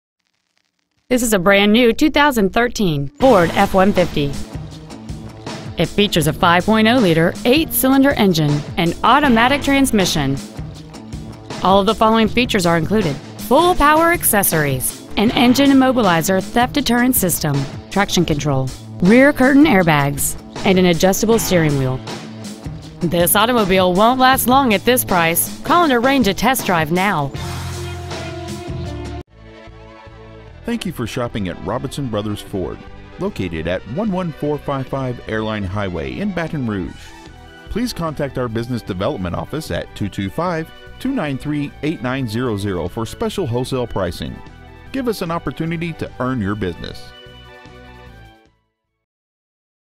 2013 Ford F-150 Baton Rouge LA
Year : 2013
 Make : Ford
 Model : F-150
 Engine : 5.0L V8
 Trans . : Automatic
 Exterior : Oxford White
 Miles : 14
 Interior : Blk Lthr Trimmed Bucket S
 Stock : 301F13